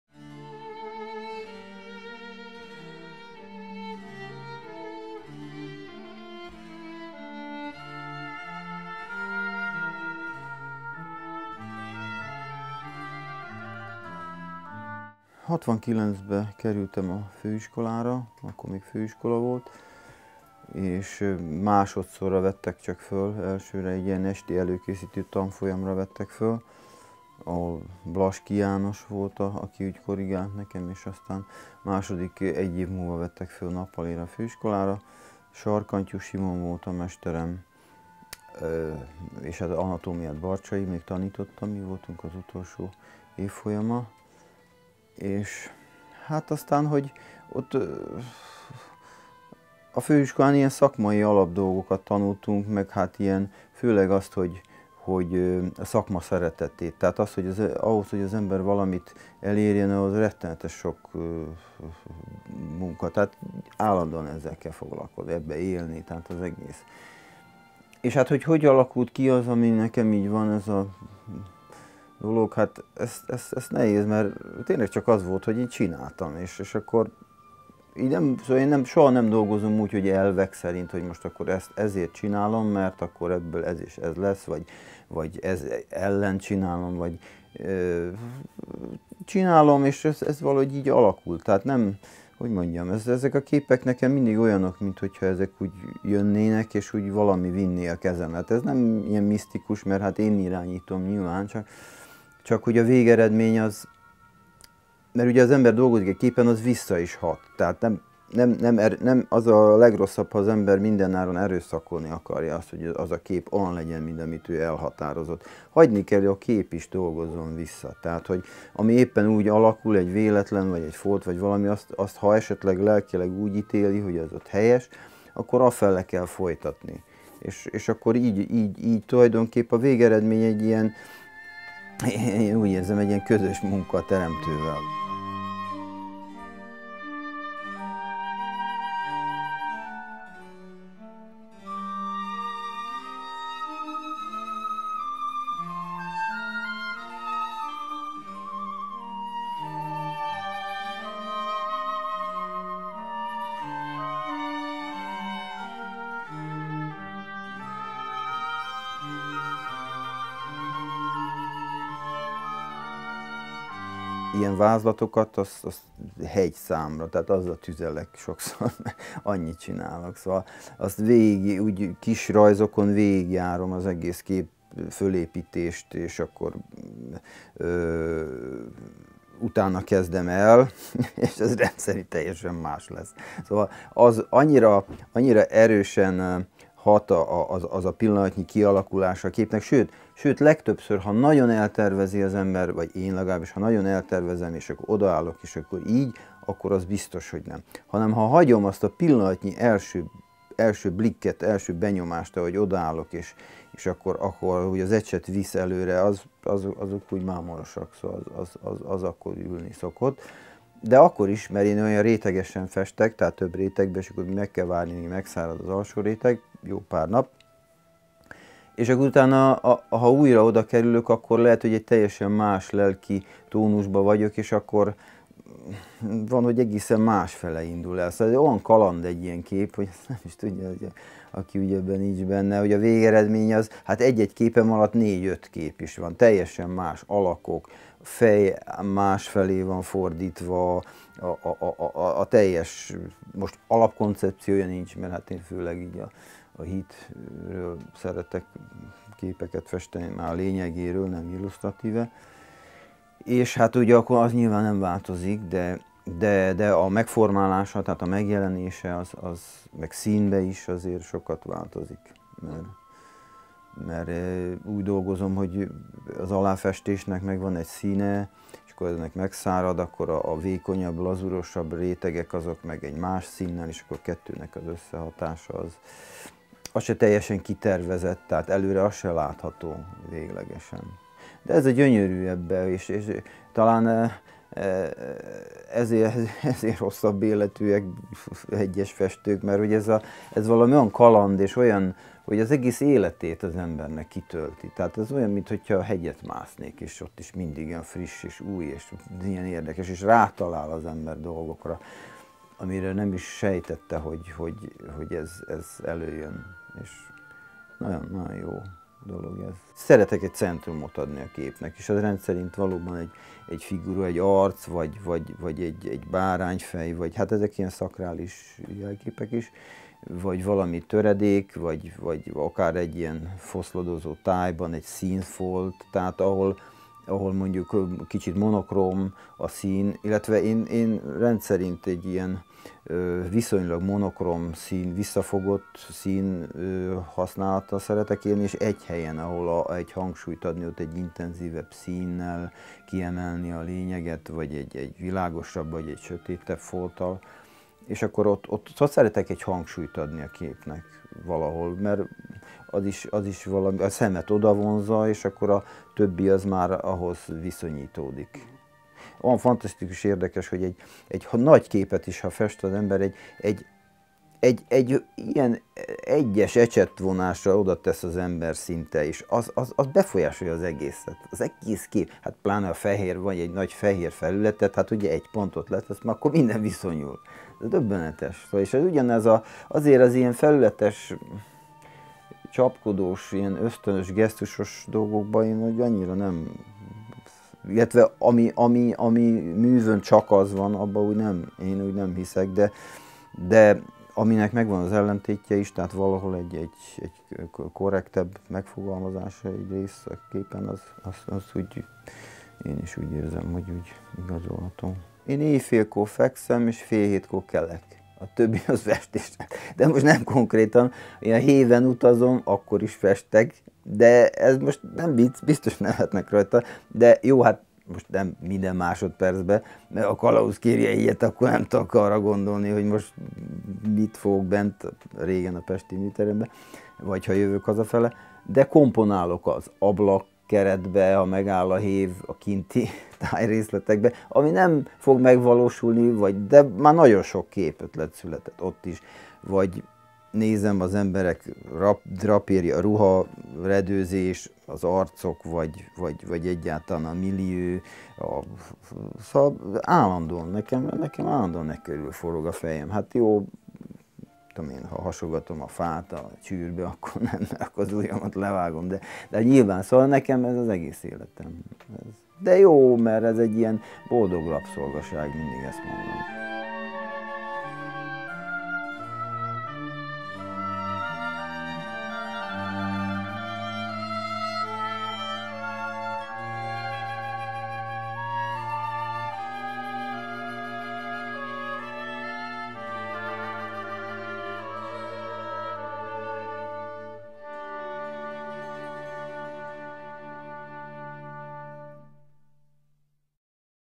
Kárpáti Tamás festőművész műtermében
Kárpáti Tamás festőművész, a Magyar Művészeti Akadémia tagja 2013-ban Kiváló Művész díjban részesült. A művész pályakezdéséről mesélt, illetve az évek során kiforrott stílusának gyökereiről és alkotói módszeréről. Még több video